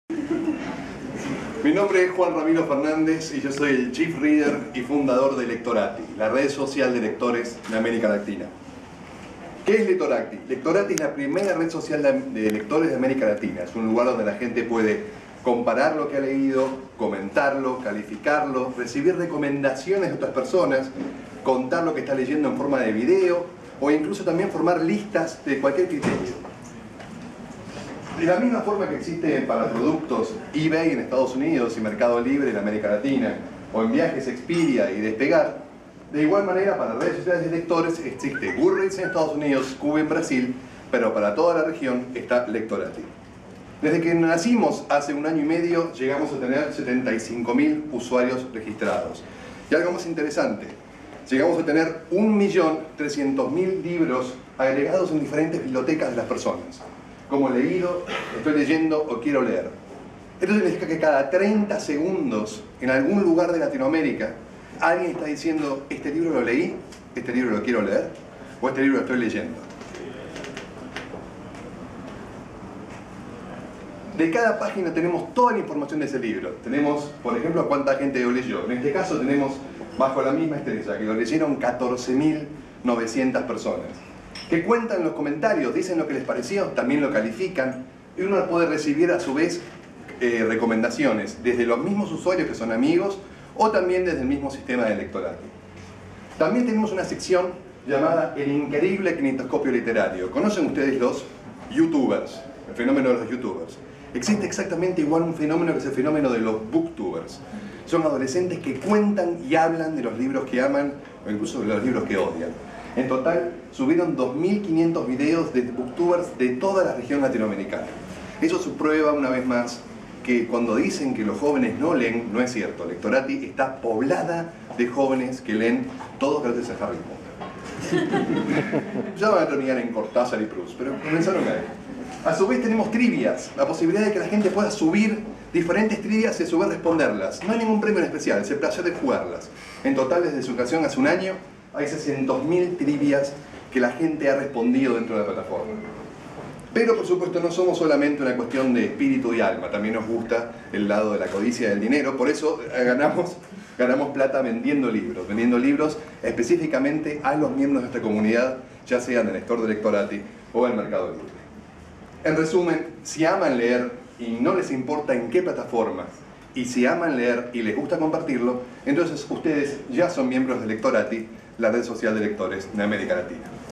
Wayra: Presentcación Lectorati
Lectorati es el primer medio social dedicado a los lectores de América Latina. Es una comunidad donde los usuarios pueden recomendar libros, comparar lo que han leído con sus amigos, crear bibliotecas de lo que han leído, y formar clubes de lectura. Tiene más de 1,5 millón de fichas de libros y más de 71 mil usuarios inscriptos, con mayor tráfico en Argentina, México y España

Seguinos en Twitter: @itenunclick, @gabybiondi.